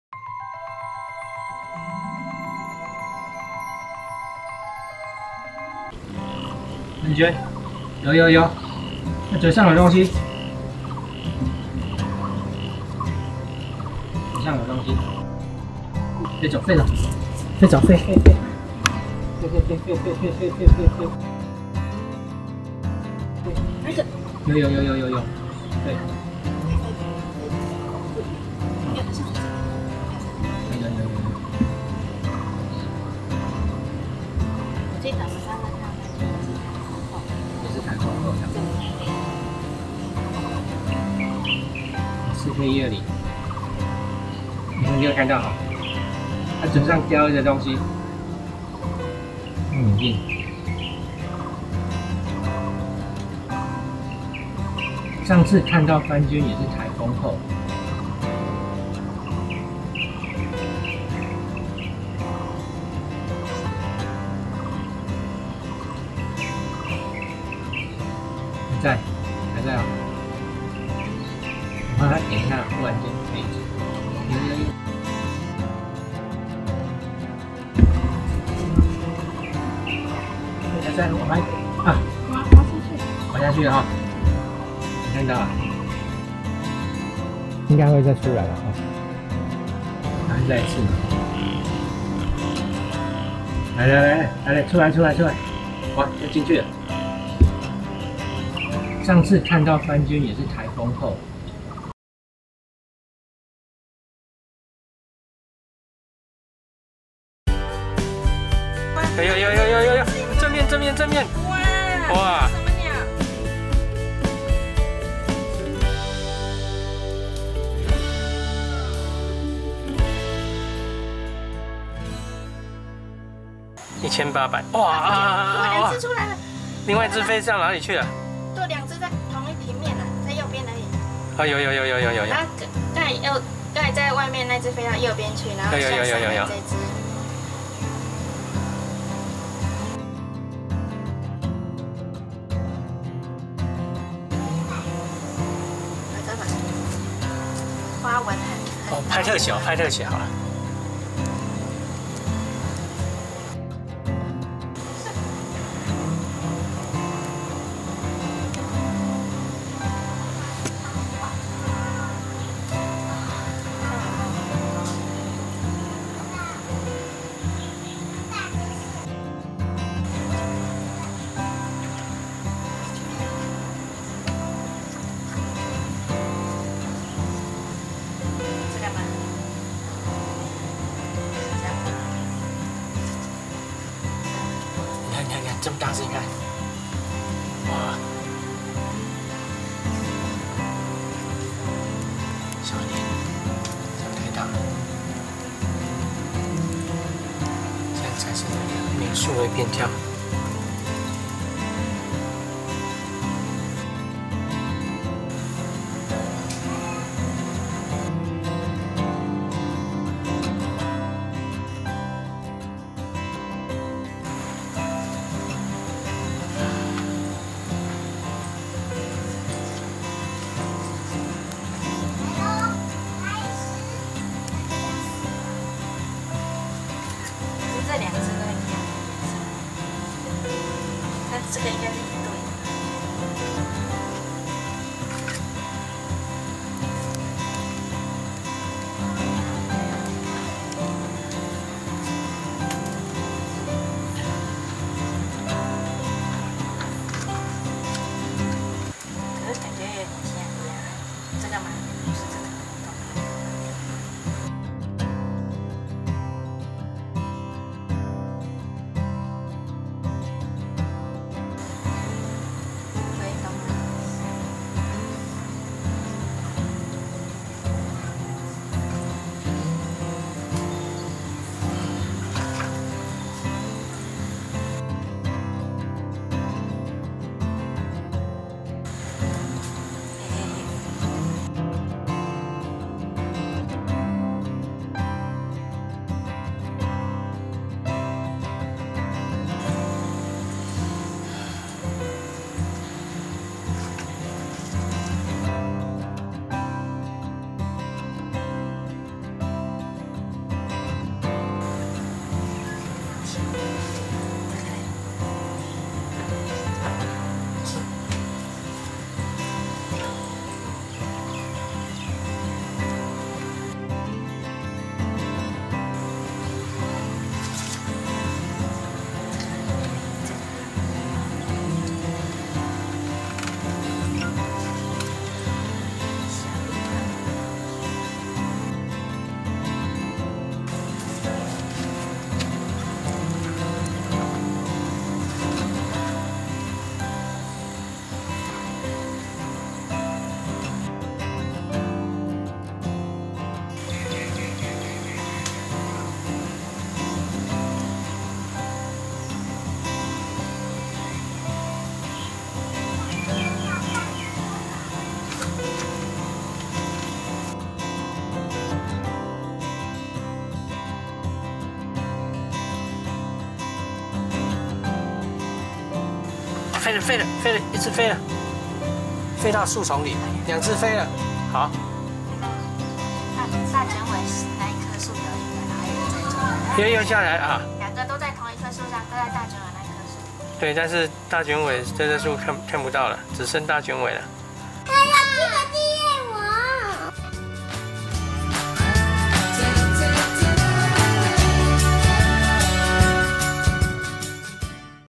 番鵑_窗外鳥世界_Centropus bengalensis_2022-09-04
2022年9月4日窗外鳥世界_番鵑
颱風過後又拍到番鵑了,
跟去年一樣都是在颱風過後拍到.

SONY A7S3 4K120
SONY 200-600mm SEL20TC